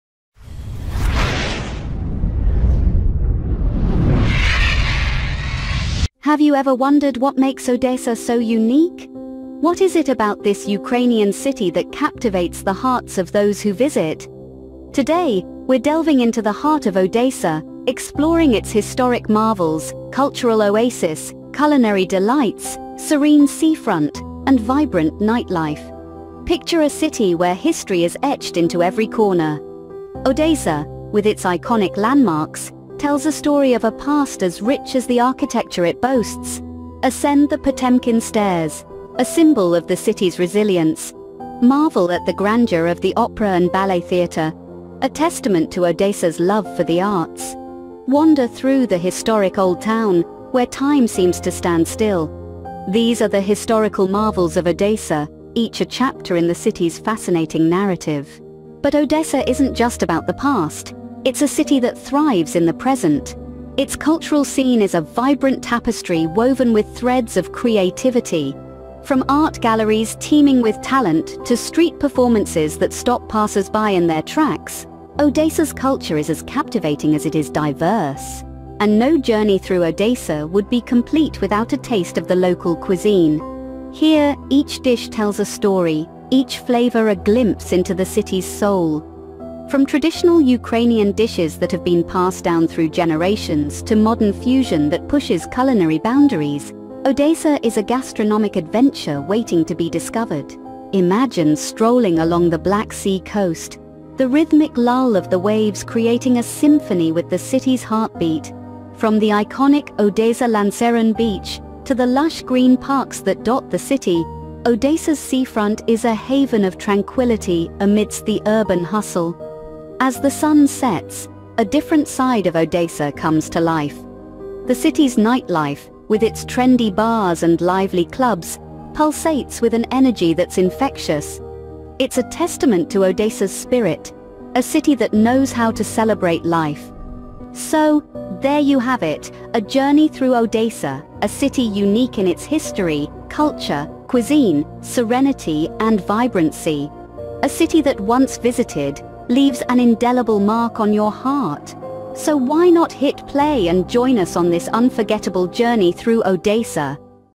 Enchanting Odesa: A Journey Through History, Culture, and Cuisine
🌟 Embark on a mesmerizing adventure through the heart of Odesa with our latest video! Join us as we explore the historic marvels, cultural oasis, culinary delights, serene seafront, and vibrant nightlife that make Odesa a truly enchanting destination. 🏰🎭🍲🌅🎉

00:00:00 - The Historic Marvels of Odesa
Step back in time as we unveil the historical treasures of Odesa, from the grand Potemkin Stairs to the majestic Opera and Ballet Theater. Discover the charm of the Old Town and witness the architectural wonders that tell the tales of Odesa's rich past.

00:00:52 - The Cultural Oasis of Odesa
Immerse yourself in the vibrant arts and cultural scene of Odesa. Visit art galleries, witness street performances, and experience the creative spirit that permeates the city. Odesa is a cultural oasis waiting to be explored!

00:01:12 - Culinary Delights of Odesa
Indulge your taste buds in the diverse and delicious culinary offerings of Odesa. From traditional Ukrainian dishes to modern fusion cuisine, Odesa's food scene is a gastronomic delight that will leave you craving for more.

00:01:34 - Serene Seafront of Odesa
Relax and take in the breathtaking views along the Black Sea coast. Explore the picturesque Odesa Lanzheron Beach, stroll through beautiful parks, and experience the tranquility that defines Odesa's serene seafront.

00:01:53 - Vibrant Nightlife of Odesa
As the sun sets, Odesa comes alive with a vibrant nightlife. Follow us as we venture into trendy bars and lively clubs, experiencing the energy and excitement that makes Odesa's nightlife truly unforgettable.

00:02:11 - Conclusion
Wrap up this enchanting journey with us and relive the beauty and magic of Odesa. Whether you're planning a visit or dreaming of future travels, Odesa awaits with open arms and a world of experiences to offer. Hit play and let the enchantment begin! 🌟